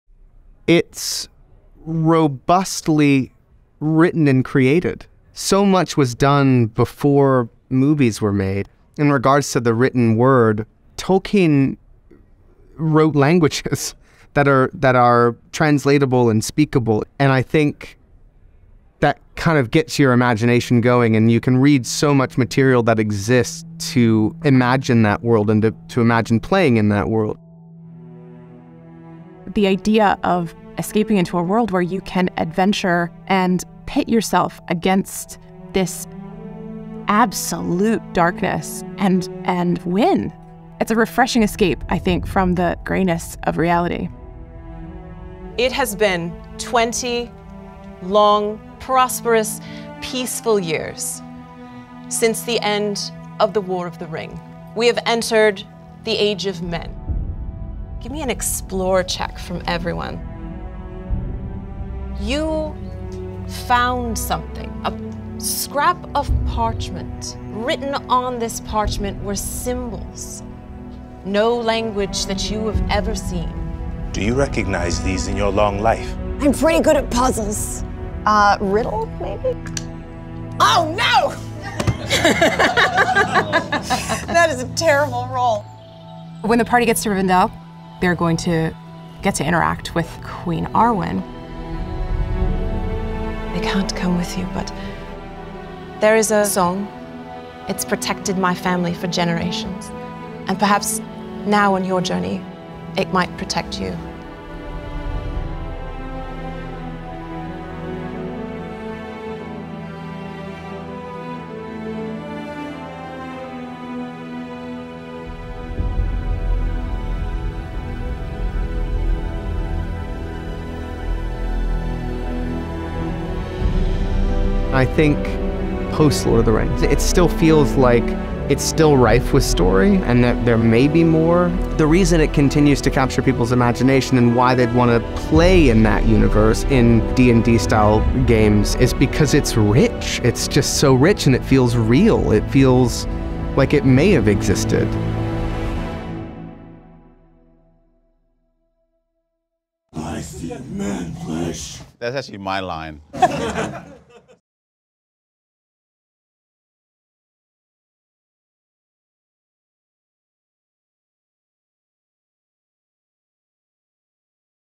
The Lord of the Rings Roleplaying Game for 5E is Out Now!
Experience the thrill of adventure in Middle-earth with the brand new The Lord of the Rings™ Roleplaying Game for 5E from Free League Publishing. Based on the award-winning second edition of The One Ring™ RPG, this adaptation features six original heroic cultures, six new classes, comprehensive rules for journeys, councils, wondrous artifacts, and the subtle magic of Middle-earth. Plus, face a host of terrifying adversaries as you seek to save the world from the returning Shadow.

To celebrate the release, check out the liveplay trailer featuring Elijah Wood, Deborah Ann Woll, Reggie Watts, and others. It includes footage from the liveplay of the game recorded by Lost Odyssey Events at the LA Comic Con.

The Lord of the Rings™ Roleplaying Game for 5E is now available. Don't miss your chance to explore Middle-earth and create your own epic tales of heroism and adventure.